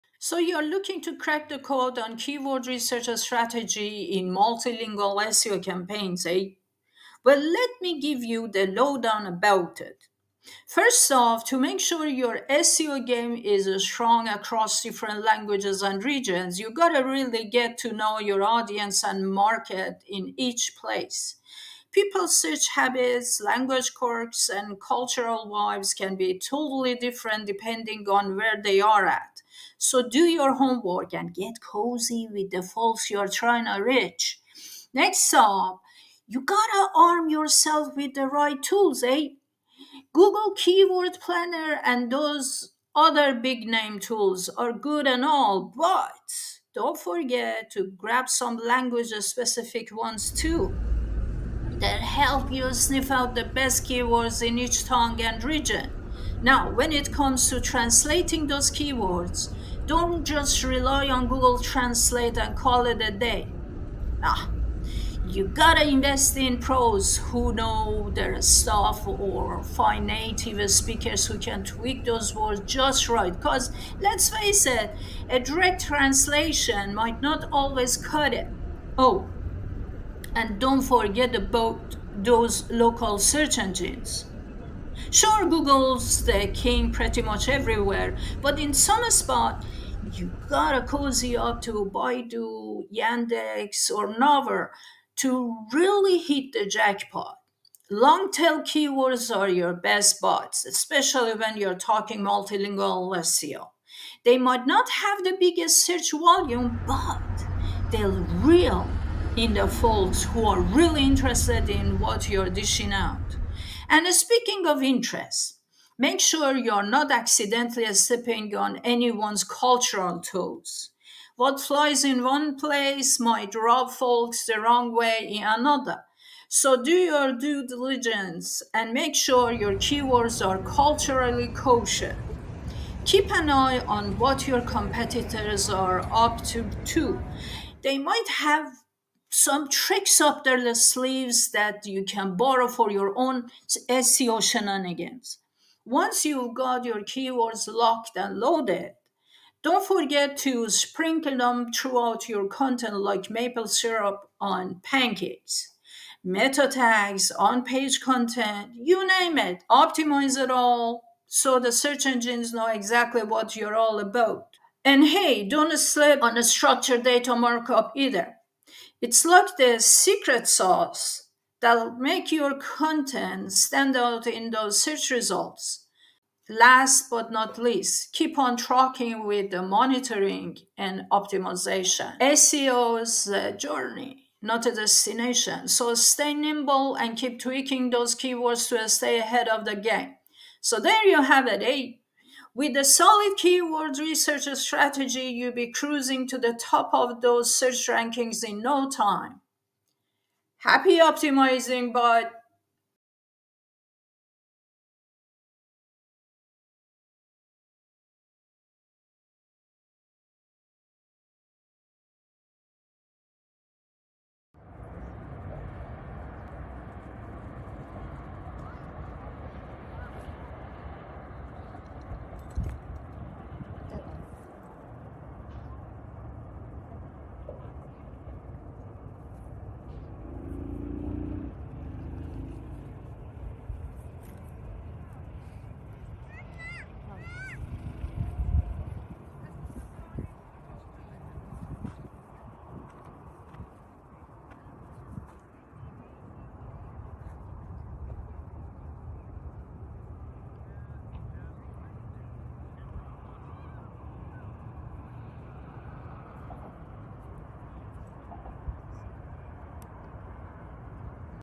Keyword Research Strategy in Multilingual SEO || Multilingual Digital Marketing
Keyword Research Strategy in Multilingual SEO? 🍁 Wanna level up your SEO game and reach folks from all corners of the globe? You're in luck, buddy! In this vid, we're spillin' the beans on how to nail that keyword research game for multilingual SEO.

From understanding what makes each audience tick to using tools that speak their language, we've got all the insider tips you need. And hey, no need to stress about direct translations—leave it to the pros or find a native speaker to make those keywords sing in every tongue.

Plus, we're not just talkin' Google here. We'll clue you in on those local search engines that can make all the diff, like Baidu, Yandex, and Naver.

And don't forget about those long-tail keywords—they're like the hidden gems of the SEO world, eh?

Oh, and cultural vibes? Yeah, they matter big time! We'll make sure you don't accidentally step on any toes with your keywords.

Keep an eye on the competition, optimize your content like a pro, and don't forget to throw in some structured data markup for good measure.

So, grab your double-double, sit back, and let's dive into this SEO adventure together! Let's do this! 🚀🍁

🌐 **Books :**